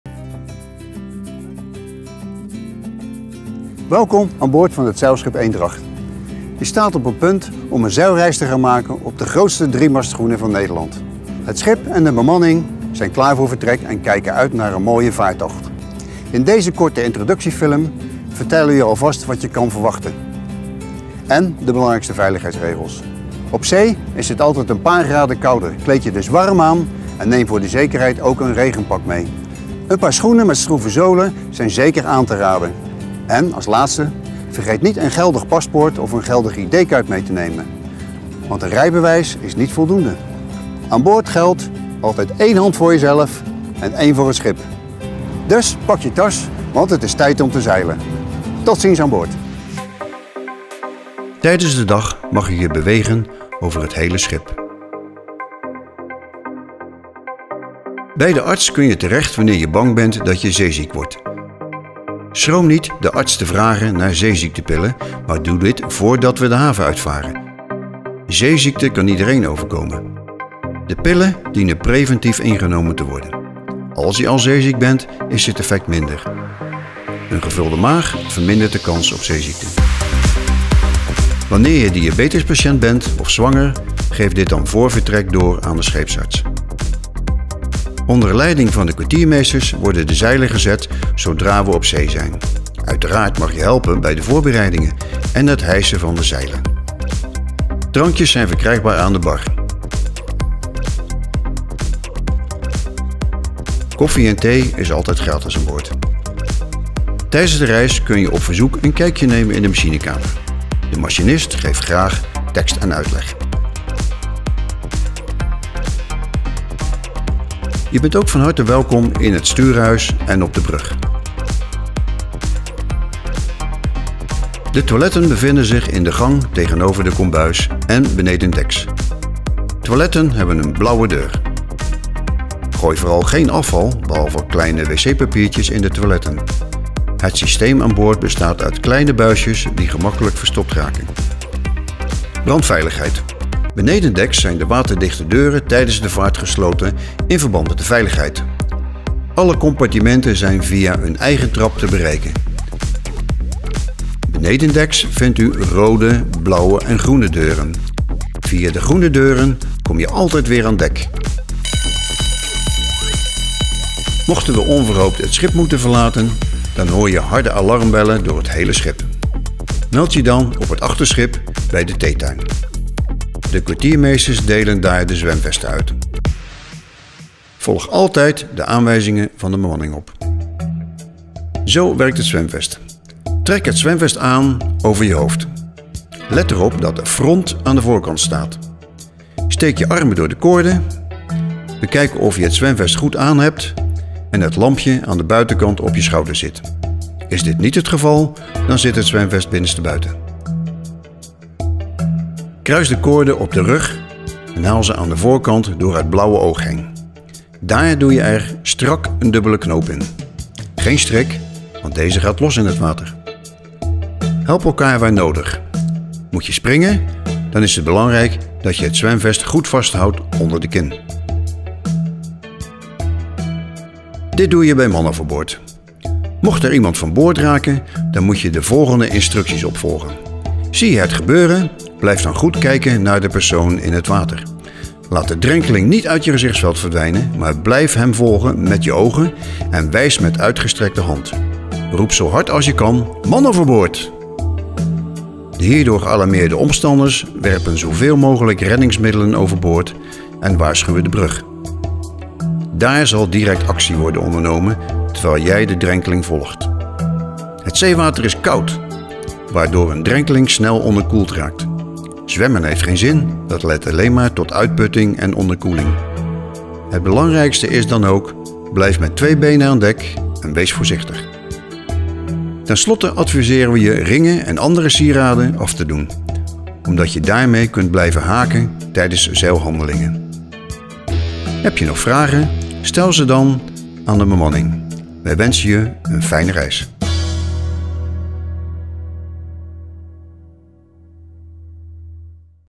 Veiligheid voorop! - veiligheidsfilm - zeilschip Eendracht
Aan boord van zeilschip Eendracht hebben wij veiligheid hoog in het vaandel staan. Bekijk deze film daarom goed en bereid je zo voor op jouw zeilreis.

- schoenen en laarzen moeten profielzolen hebben
- tijdens werkzaamheden (dus tijdens de wacht) geen sieraden dragen zoals ringen, armbanden, halskettingen, horloges en oorbellen.
- draag een brillenkoordje aan de bril
- vertel het de scheepsarts als je medicijnen gebruikt
- in verband met de veiligheid aan boord: alcoholgebruik met mate en nooit tijdens de wacht
- drugs, ook softdrugs zijn absoluut verboden te gebruiken of in het bezit te hebben
- waterdichte deuren moeten op zee gesloten blijven
- in de hutten is er voor iedereen een zwemvest aanwezig dat is goedgekeurd door de scheepvaartinspectie
- er zijn speciale regels voor het aan dek zijn in het donker, die je bij aanvang van de reis worden medegedeeld.